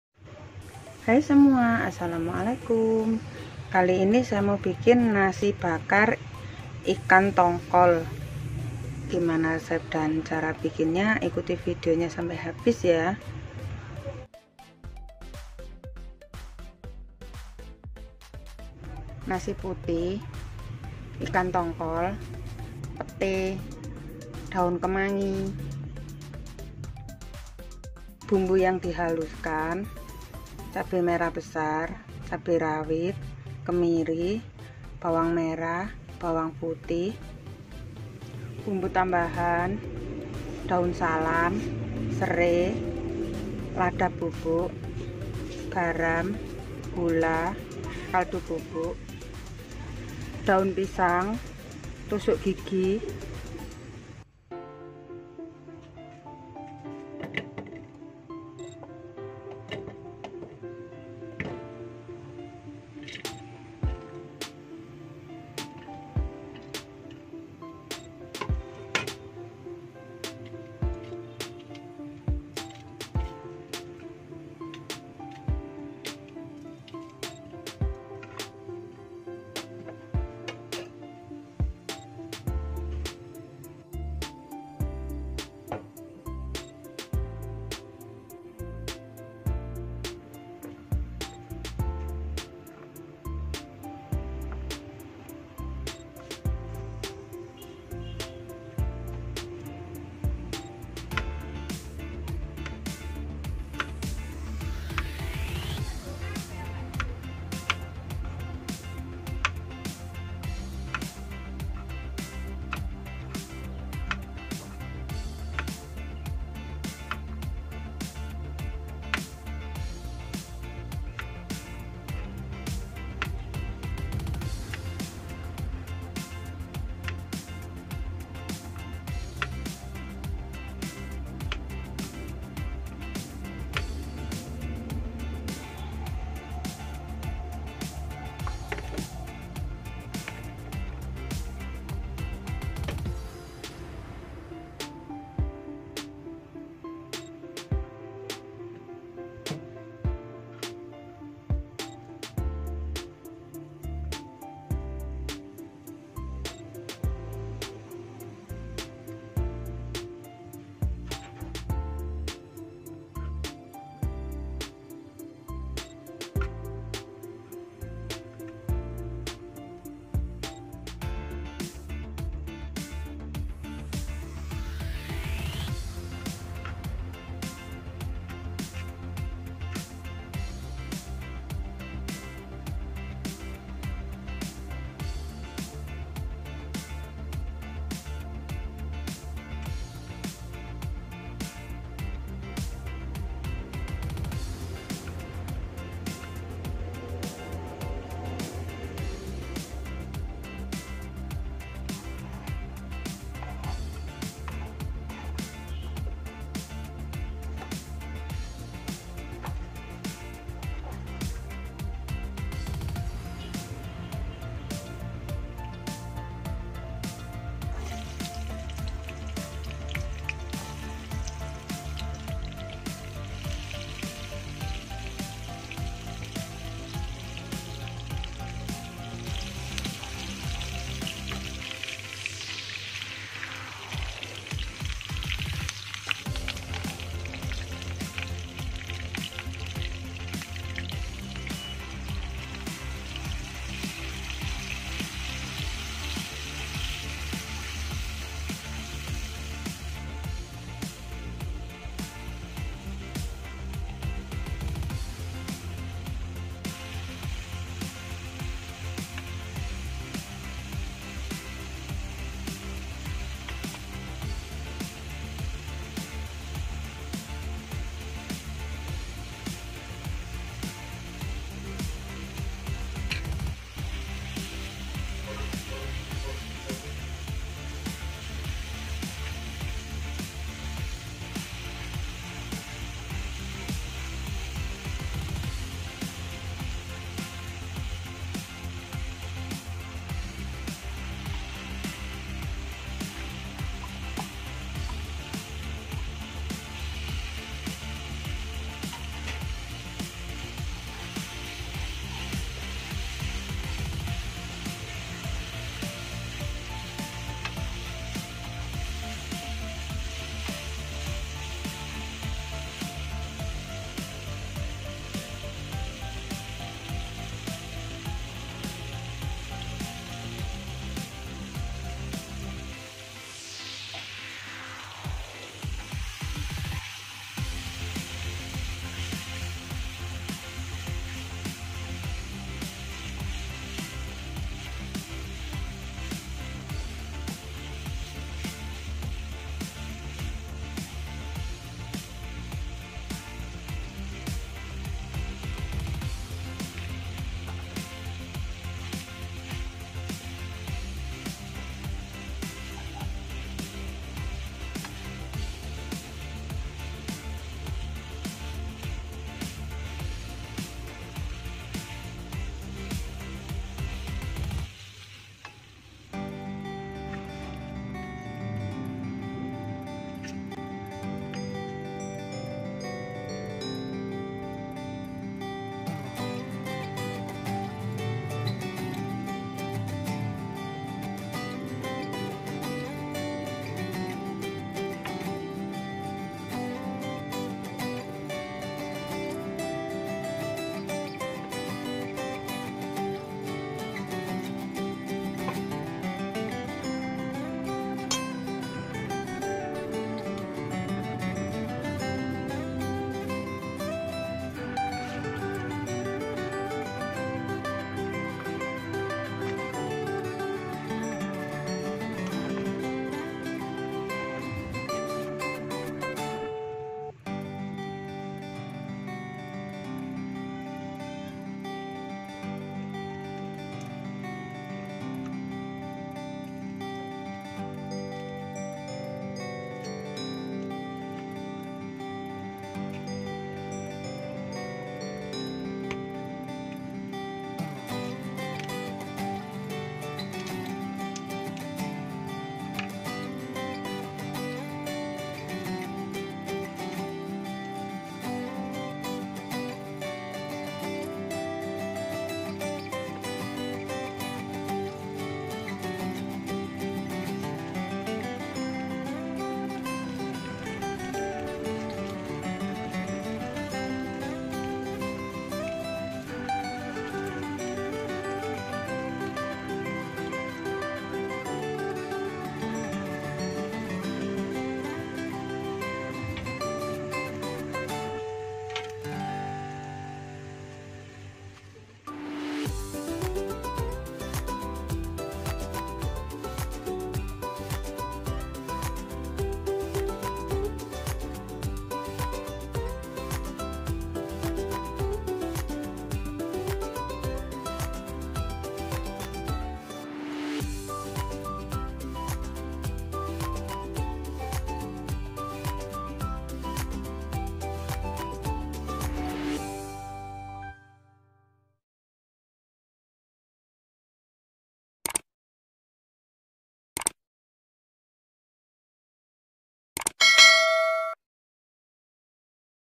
NASI BAKAR IKAN TONGKOL // Kemangi & Pete Enaknya Kebangetan
Ndut TV Official

Makasih buat kalian semua yang sudah nonton video kita.

NASI BAKAR IKAN TONGKOL
(Kemangi & Pete)

Nasi putih secukupnya
2 ekor ikan tongkol (ukuran sedang)
2 papan pete
1 genggam daun kemanggi

Bumbu halus :
2 siung bawang putih
7 siung bawang merah
2 biji cabe merah besar
4 butir kemiri
10 biji cabe rawit

Bumbu tambahan:
1 batang serai
3 lembar daun salam
1sdt garam
1 sdt kaldu bubuk
1/2 sdt gula pasir

Daun pisang & tusuk gigi secukupnya

TERIMA KASIH
SELAMAT MENCOBA